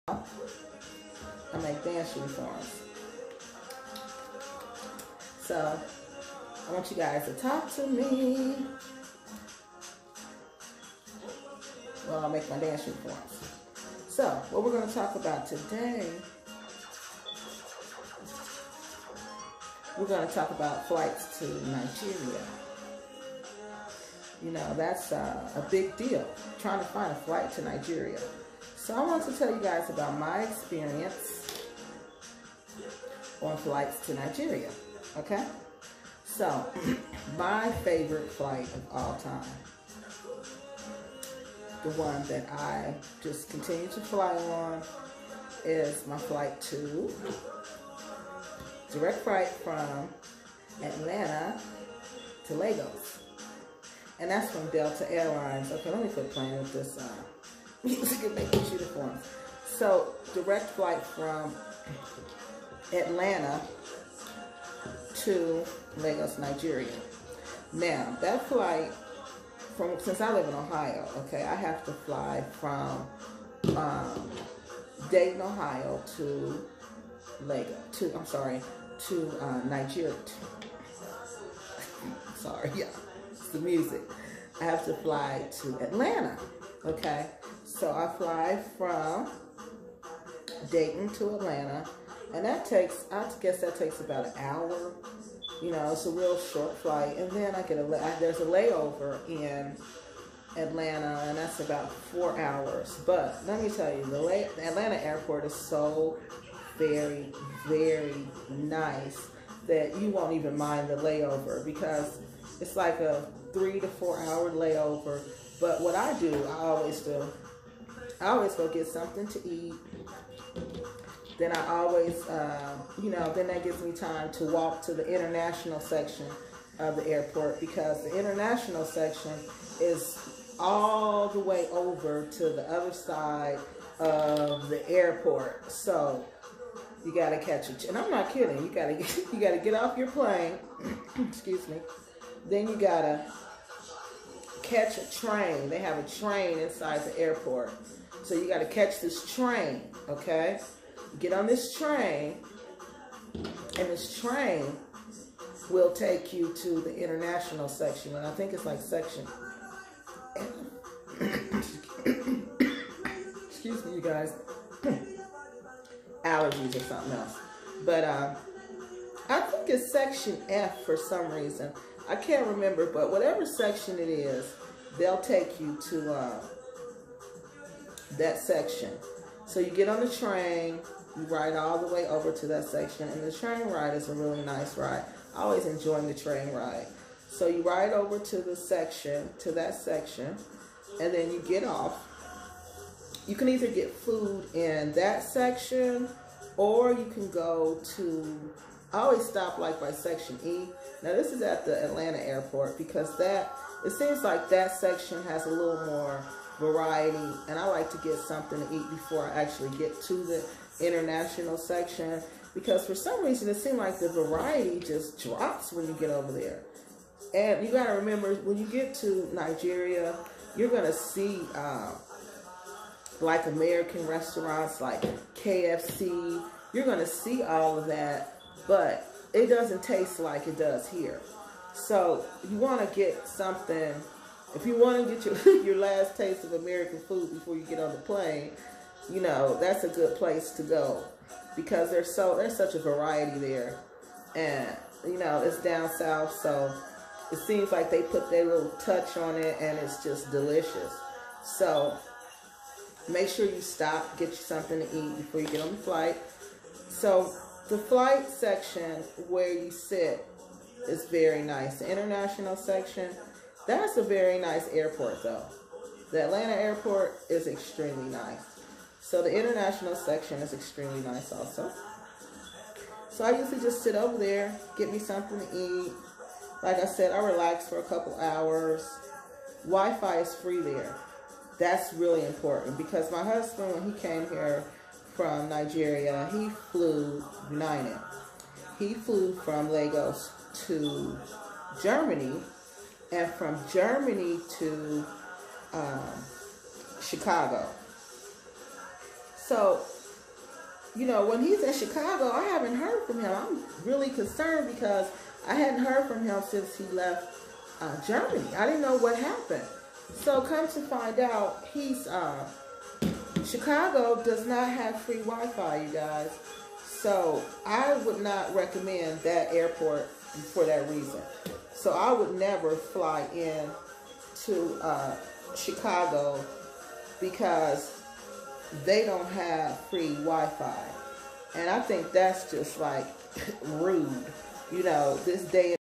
Let's TALK about DIRECT FIGHTS to Lagos, NIGERIA PART ONE ❤💯💃🛩
In  This video I talk about direct flights to Lagos Nigeria. Specifically   Delta Airlines  fight 53❤